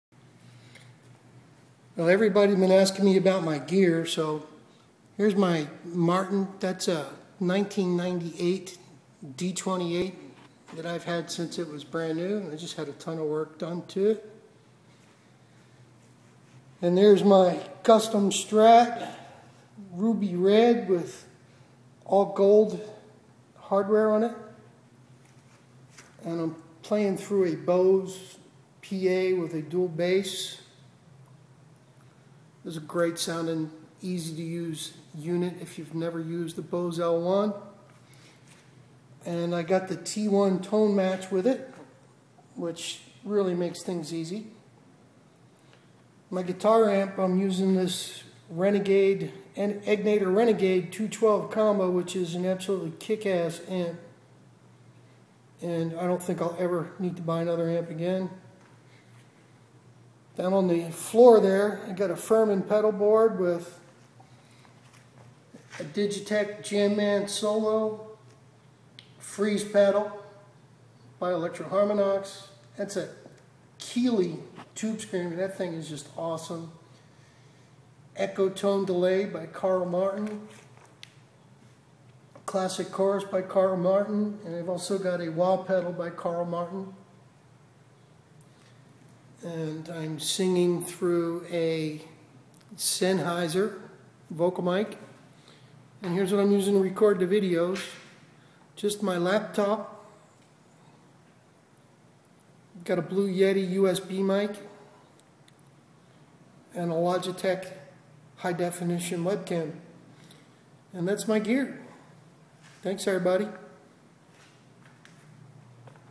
Here is a video of my gear
Here is a gear list for ya,

Guitars :  1998 Martin D28 with Martin Marquis 12 , Fender custom shop hot rod with Fender Lace Pickups

PA: Bose L1 with Tone match and double bass  , Sennheiser E945 MIc

Amp : egnater Renegade 212 combo

effects, Furman powered pedal board , digitech Jamman solo, Freeze Pedal by electro harmonix, Keeley ibanez tube screamer, carl martin echotone, carl martin classic chorus , carl martin wah

recording : Blue yeti usb mic, logitech hd camera and laptop